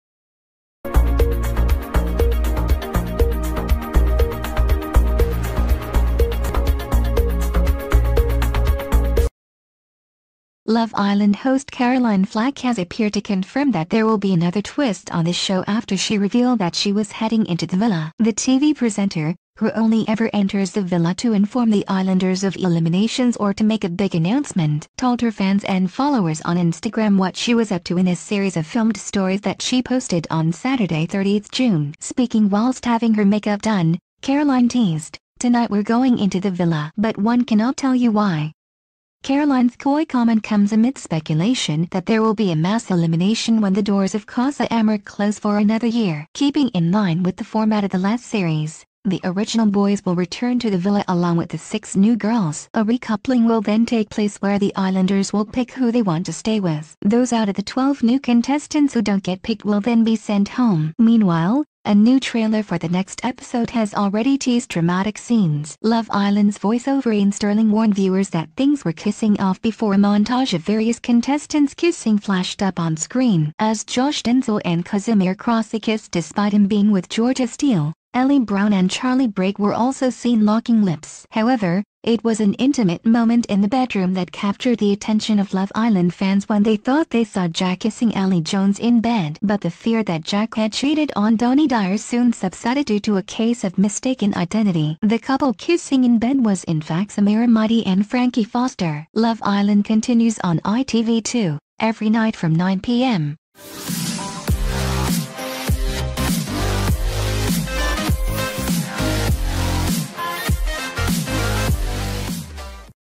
Caroline Flack 'confirms' latest Love Island twist amid speculation of multiple eliminations during
Caroline Flack 'confirms' latest Love Island twist amid speculation of multiple eliminations during stick or twist recoupling
Love Island host Caroline Flack has appeared to confirm that there will be another twist on the show after she revealed that she was heading into the villa.
The TV presenter, who only ever enters the